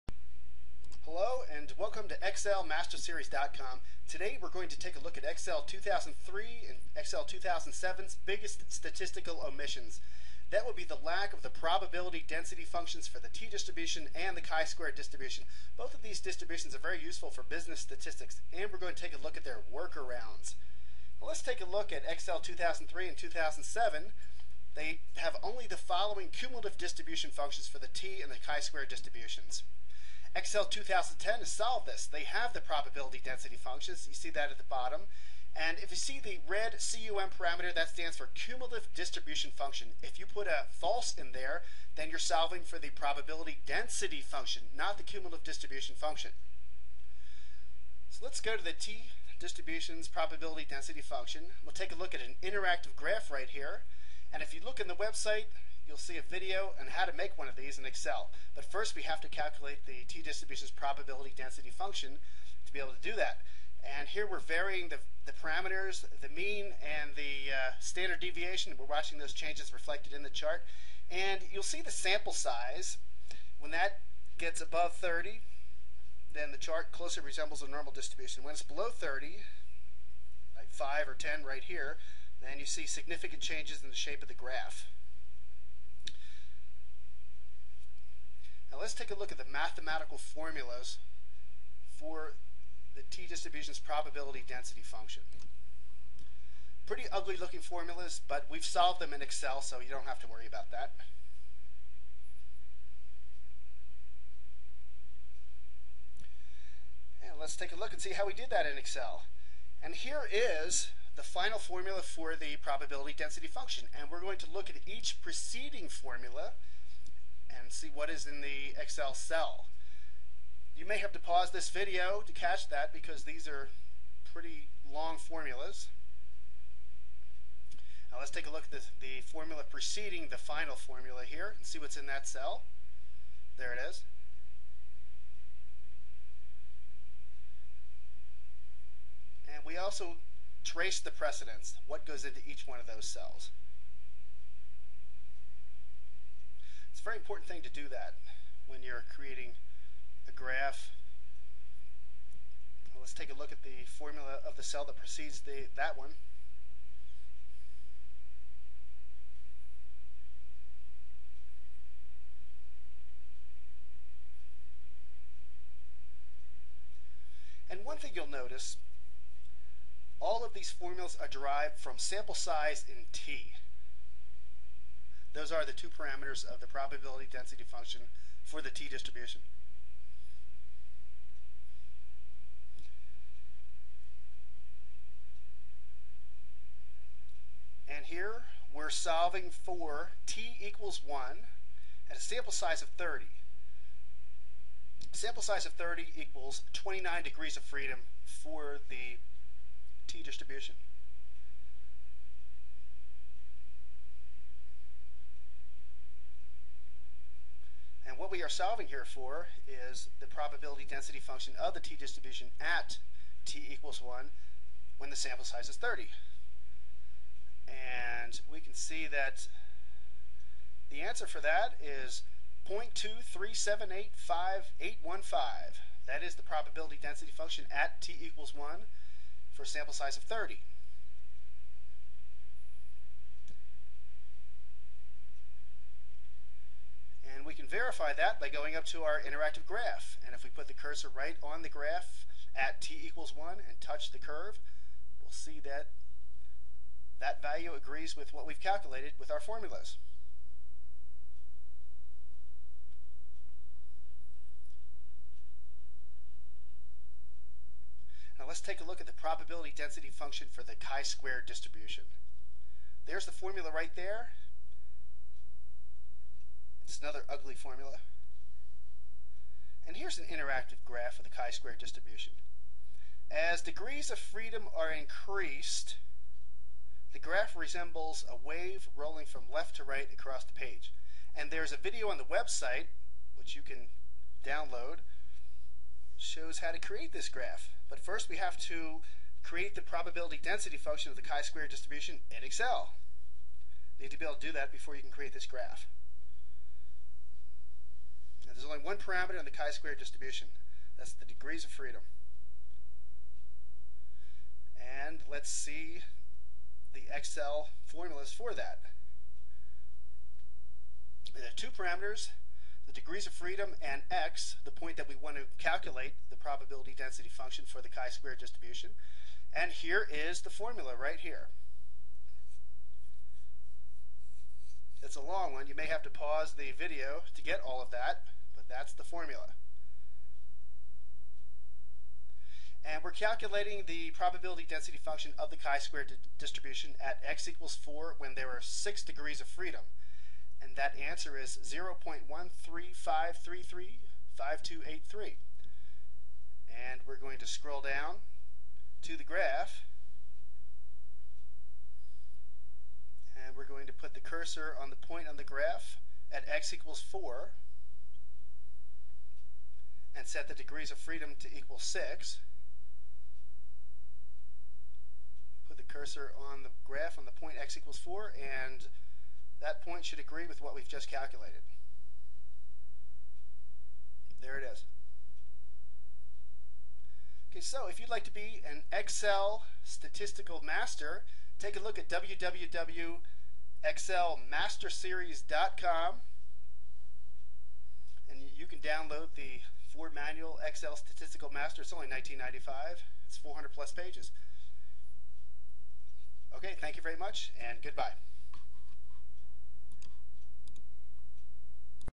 The Biggest Statistical Omissions of Excel 2003 and 2007
Here are the Excel Work-Arounds for the Two Biggest Statistical Omissions in Excel 2003 and 2007. These are the Probability Density Functions of the t Distribution and the Chi-Square DIstribution.